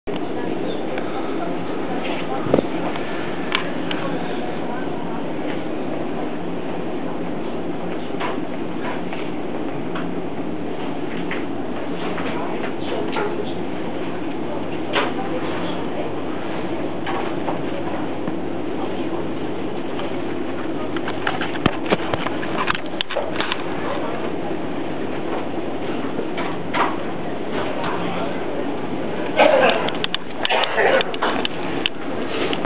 Camel Smoking Lounge Zurich Switzerland Airport December 2010
CAMEL SMOKING
"smoking seriously damages your heath"

fishtank of smokers in zurich airport. weird mix of privilege and shame, advertising both a brand and the act as a voyeuristic spectacle. had to check it out.